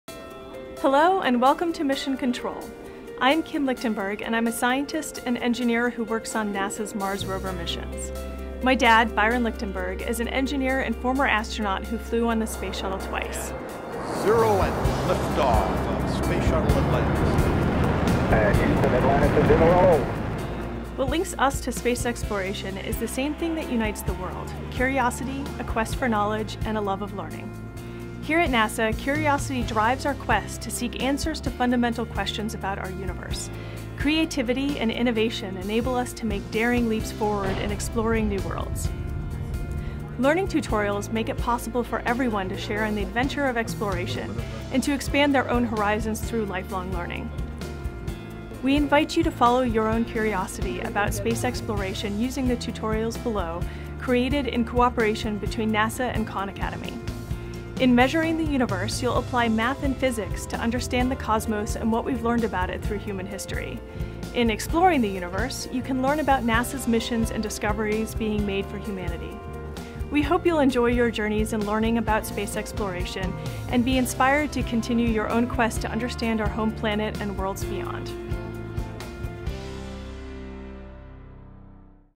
Khan Academy & NASA Introduction
Welcome to NASA Mission control, an introduction to the learning and exploration adventures offered in cooperation between NASA and Khan Academy!